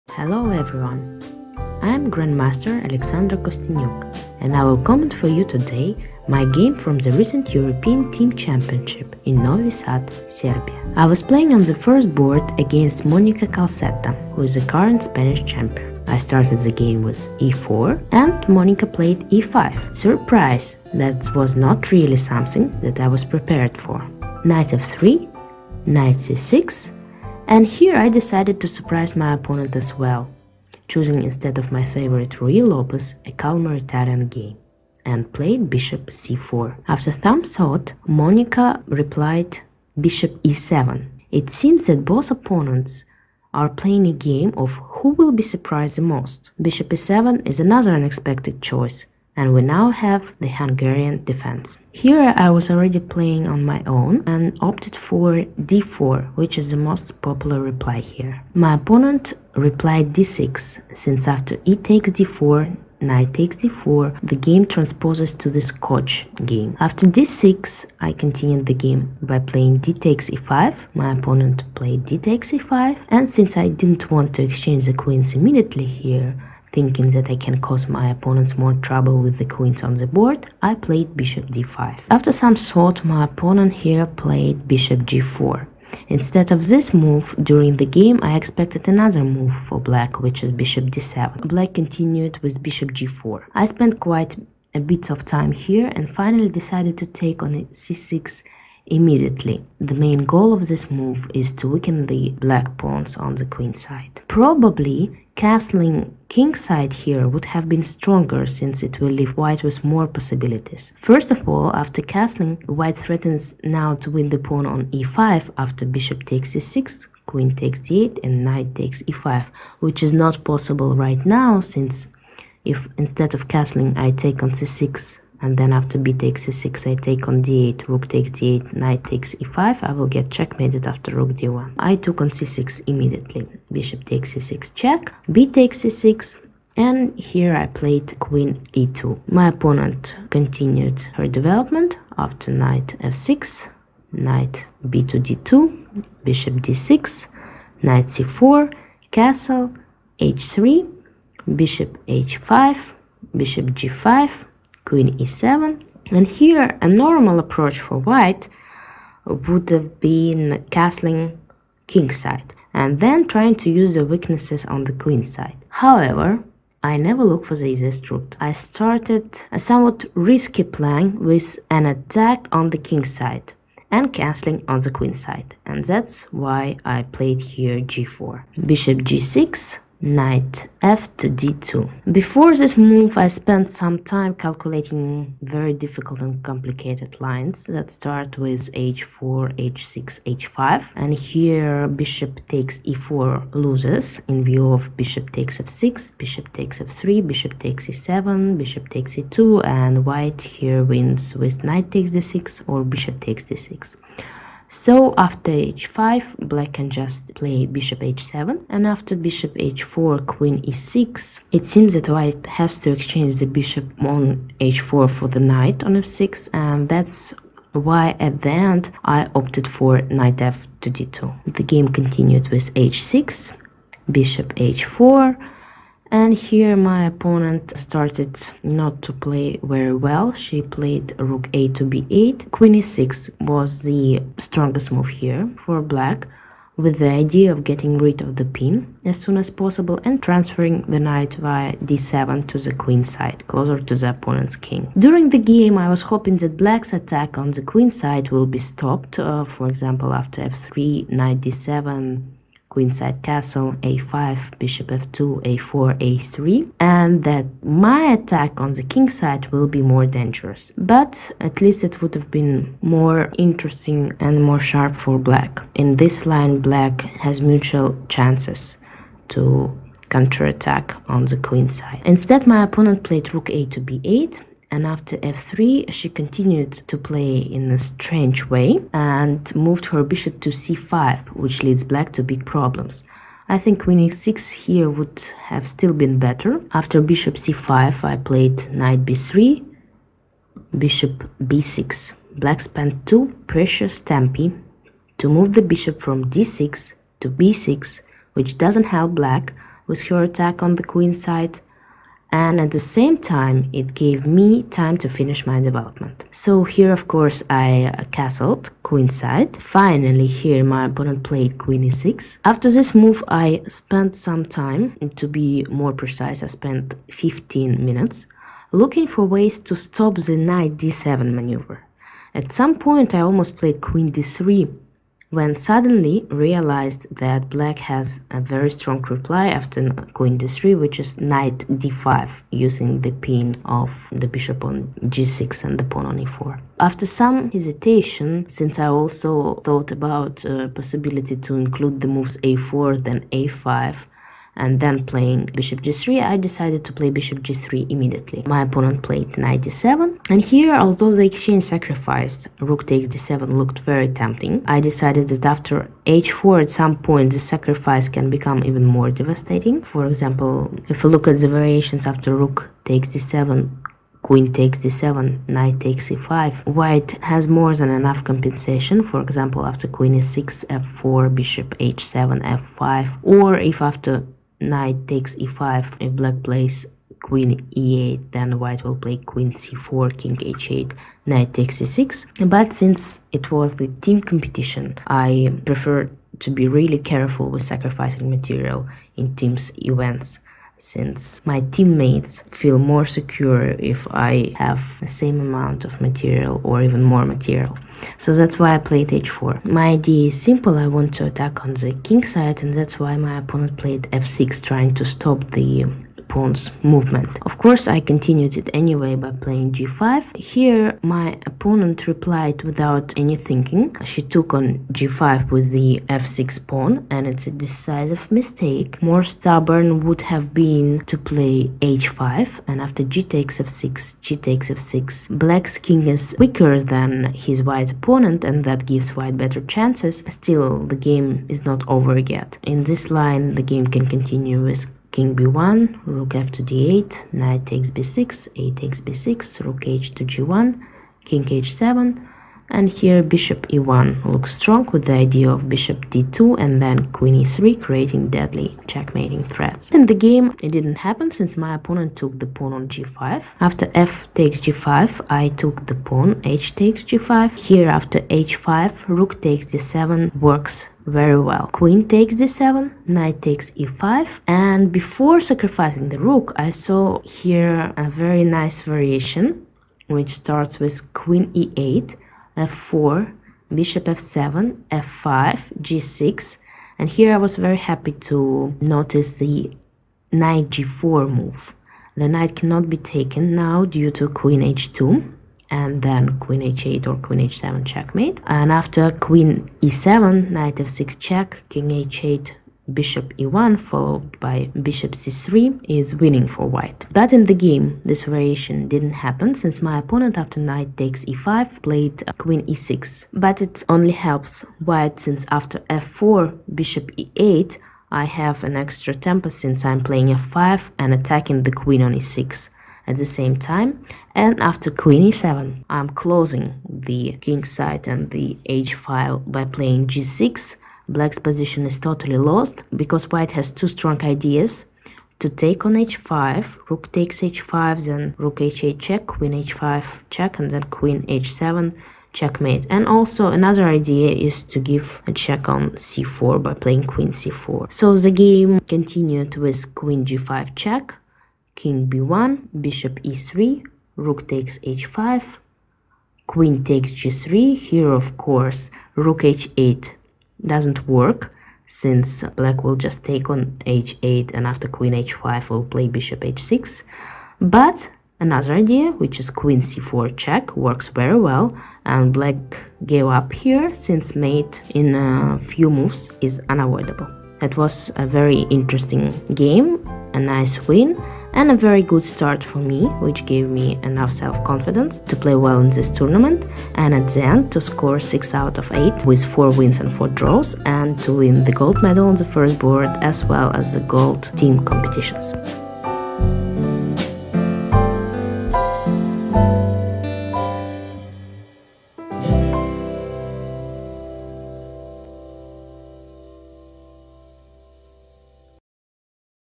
Kosteniuk - Calzetta, Novi Sad 2009 European Chess Champ.
This is the game from the European Team Championships in Novi Sad between women's world chess champion and chess queen Alexandra Kosteniuk and Spanish champion Monica Calzetta. It was played on the first board of the encounter Russia - Spain.